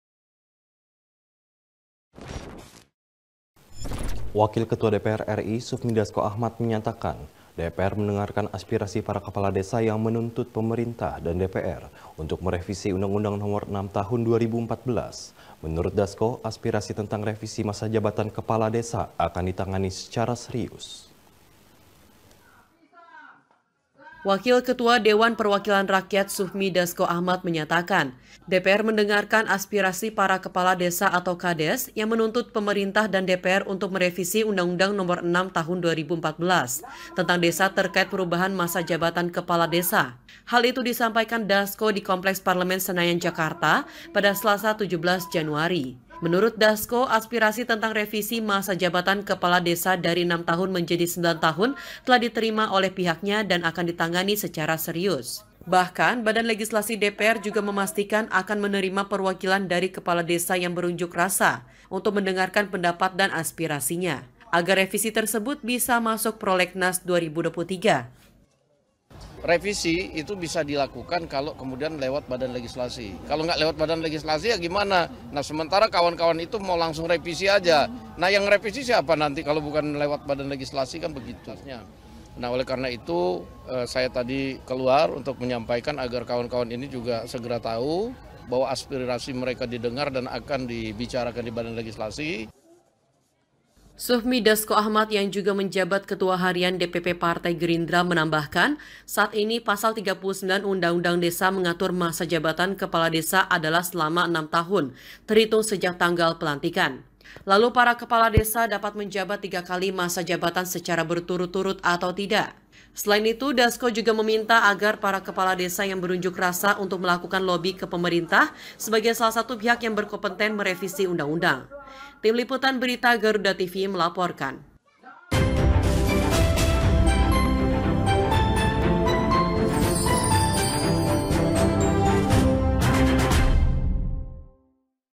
Sufmi Dasco Ahmad DPR Serap Aspirasi Kades Terkait Masa Jabatan Kepala Desa
Wakil Ketua DPR RI Sufmi Dasco Ahmad menyatakan DPR mendengarkan aspirasi para kepala desa yang menuntut pemerintah dan DPR untuk merevisi undang – undang nomor 6 tahun 2014. Menurut Dasco aspirasi tentang revisi masa jabatan kepala desa akan ditangani secara serius.